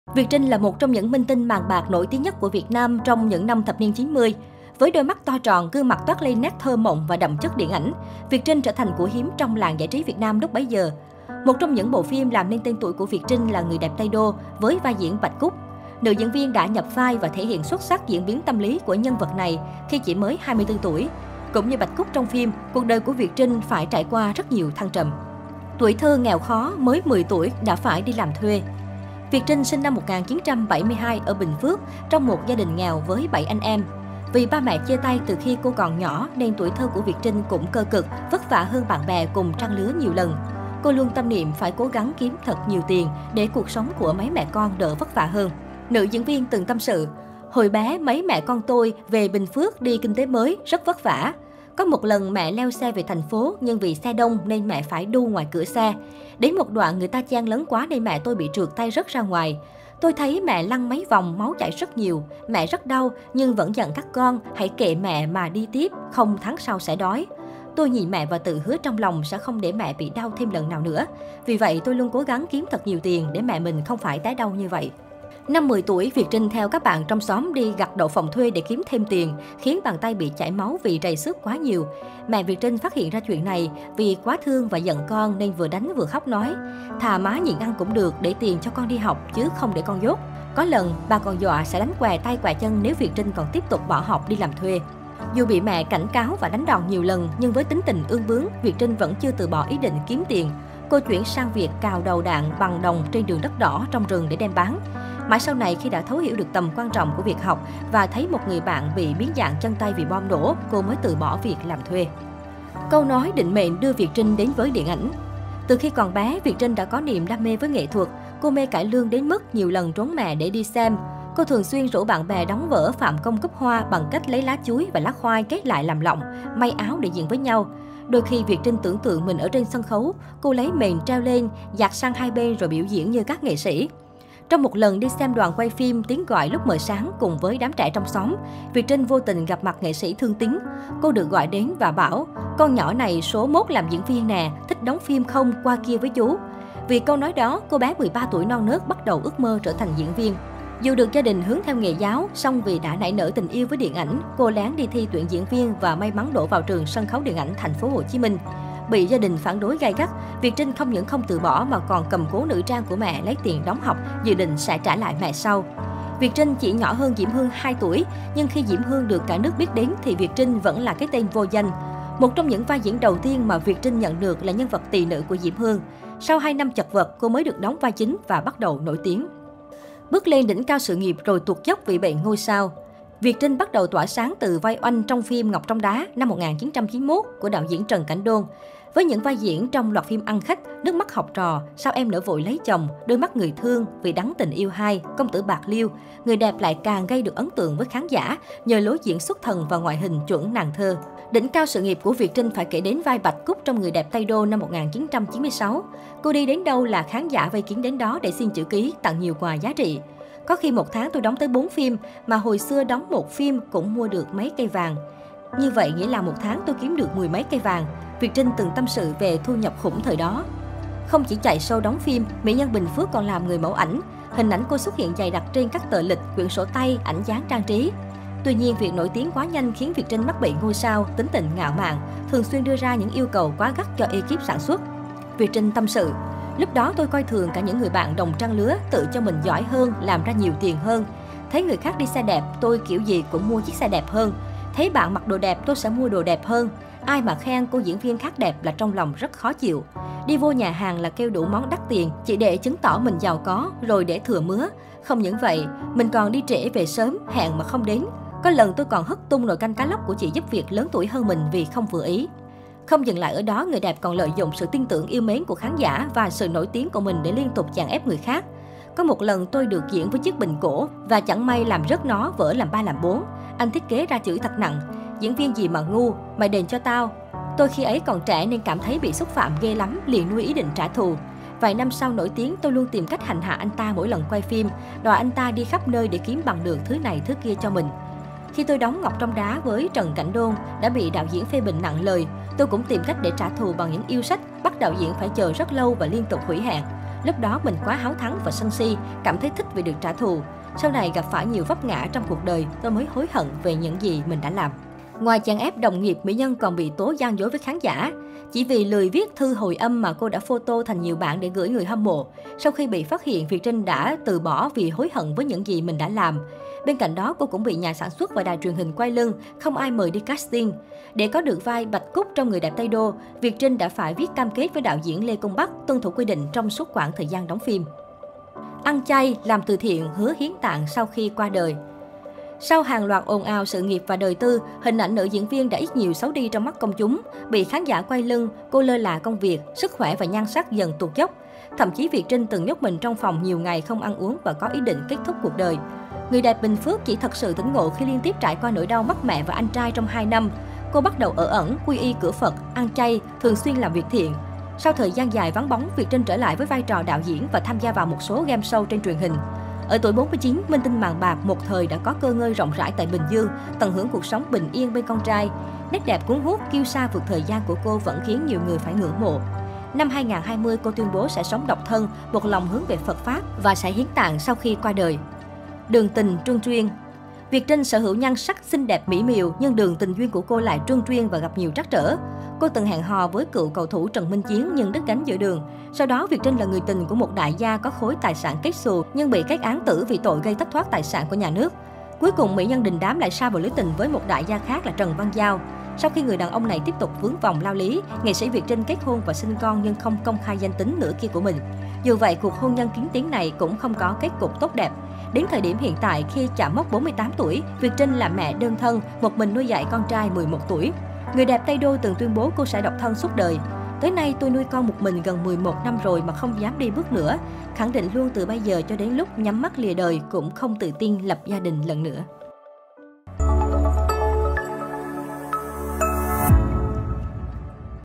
Việt Trinh - “Người đẹp Tây Đô” tụt dốc sự nghiệp, trả giá đắt vì chảnh chọe, chèn ép đồng nghiệp
Việt Trinh là một trong những minh tinh màn bạc nổi tiếng nhất của Việt Nam trong những năm thập niên 90. Với đôi mắt to tròn, gương mặt toát lên nét thơ mộng và đậm chất điện ảnh, Việt Trinh trở thành "của hiếm" trong làng giải trí Việt Nam lúc bấy giờ.